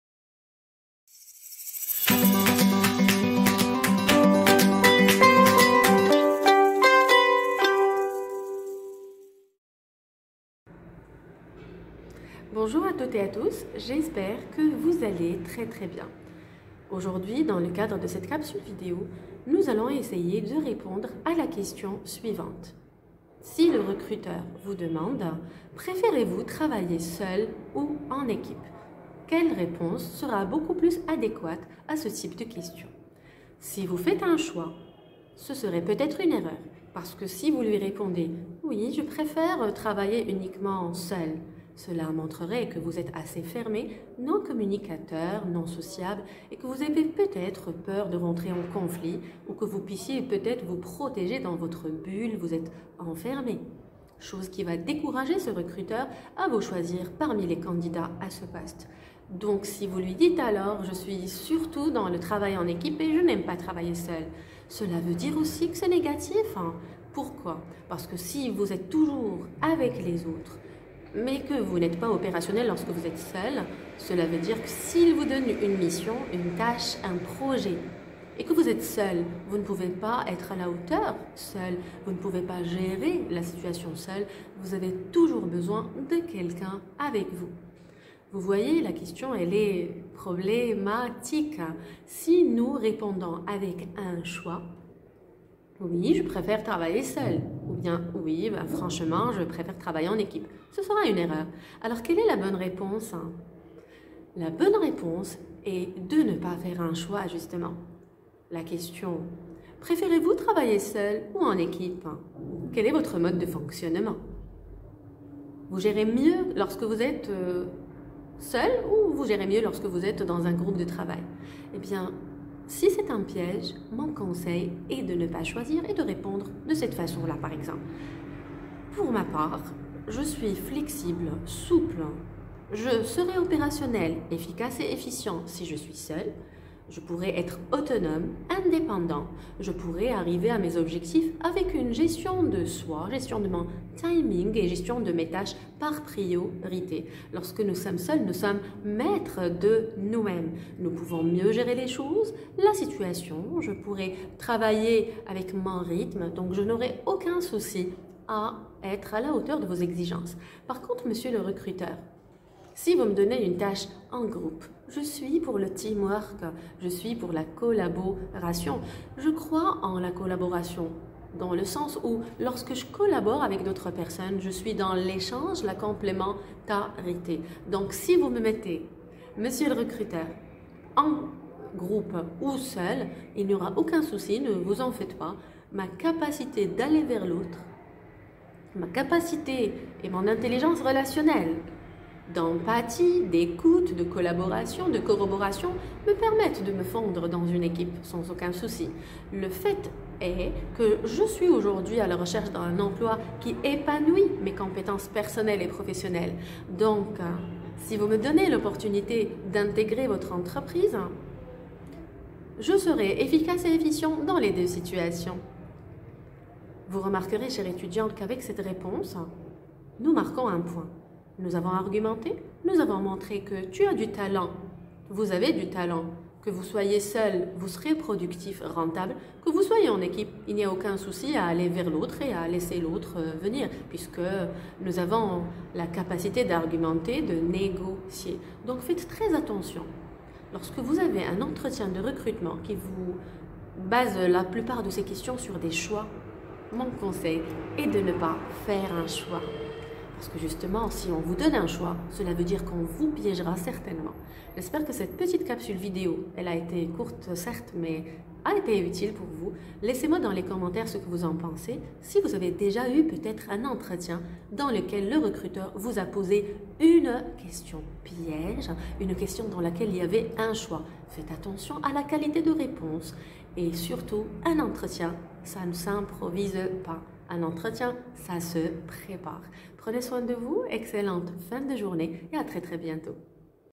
Préférez-vous travailler seul ou en équipe? Question de l'Entretien d'Embauche/ Pr Soumaya DLIMI
Souvent, dans le cadre d'un Entretien de Recrutement, le recruteur peut poser la question suivante: Préférez vous travailler seul ou en équipe??? il s'agit d'un piège! regardez cette vidéo et découvrez la réponse!
je vous souhaite une excellente semaine, pleine de courage et de motivation. Laissez moi dans la partie commentaire votre expérience passée dans le cadre des entretiens  d'embauche.